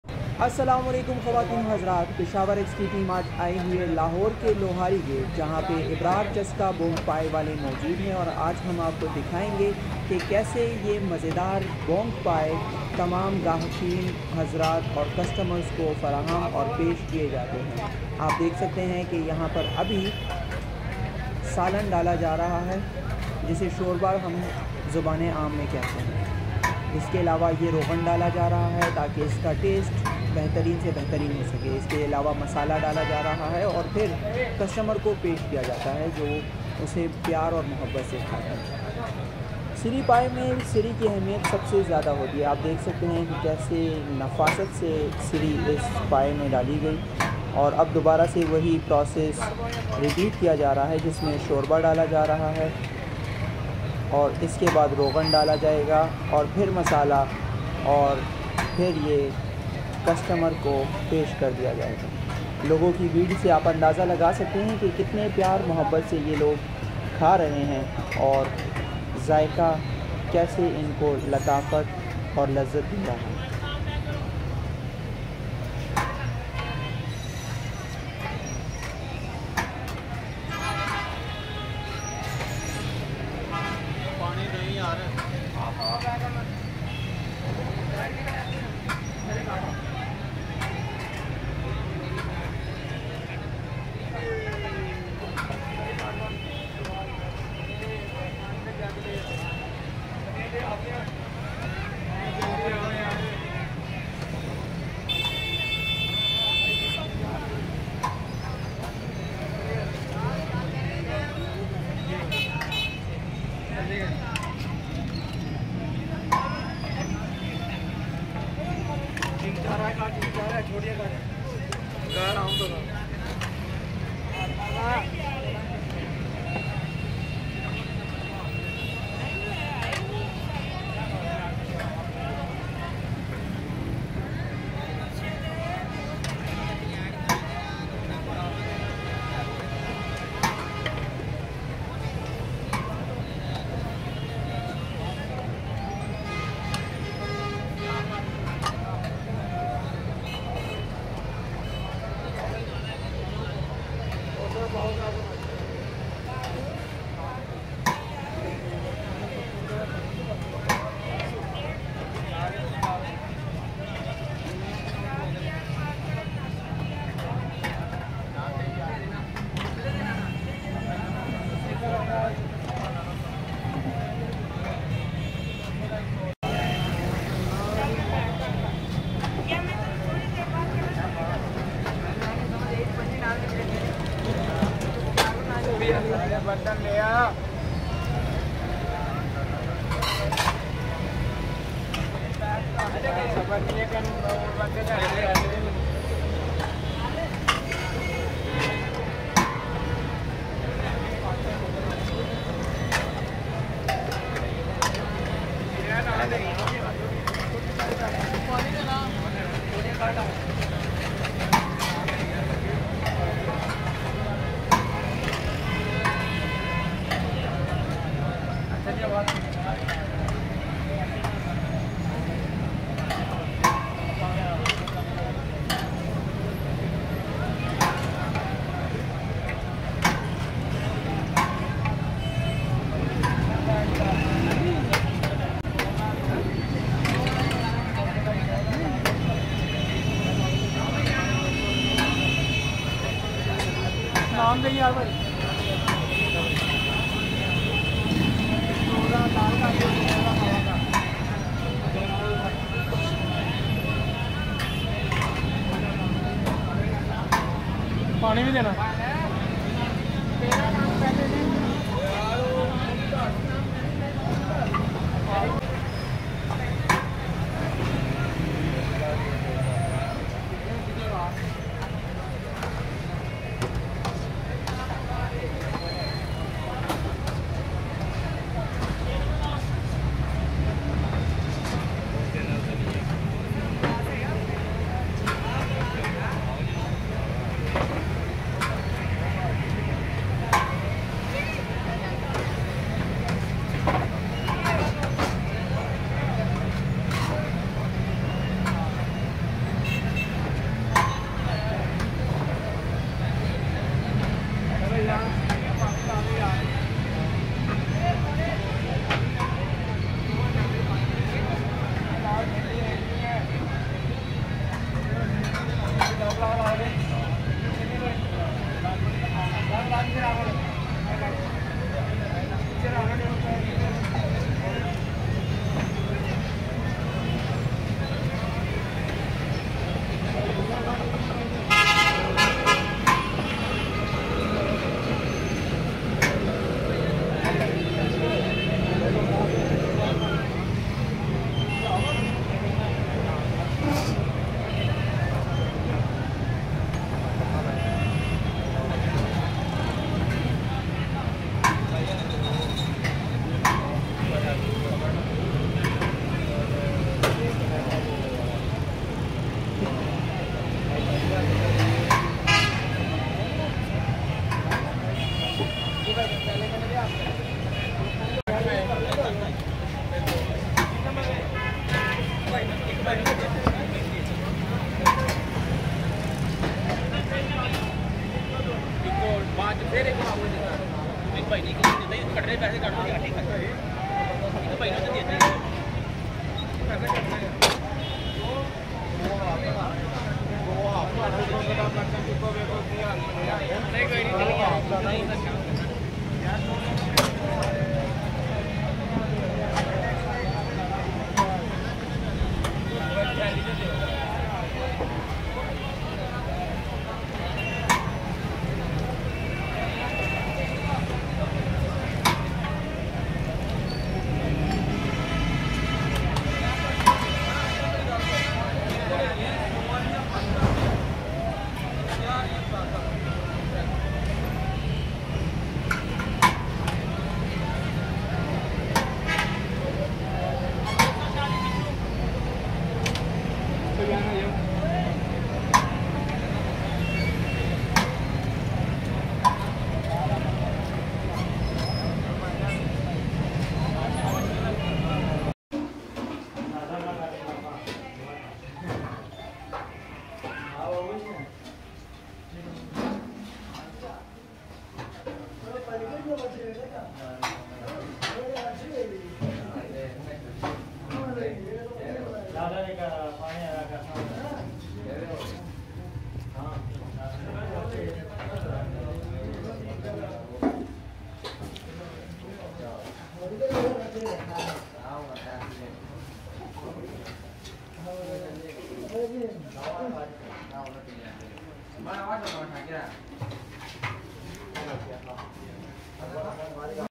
Street Food in Lahore - Ibrar Siri Paye, Lohari Gate Lahore | Lahori Nashta
Location: Ibrar Chaska Bong Paye - Near Muslim Masjid Lohari Gate Lahore.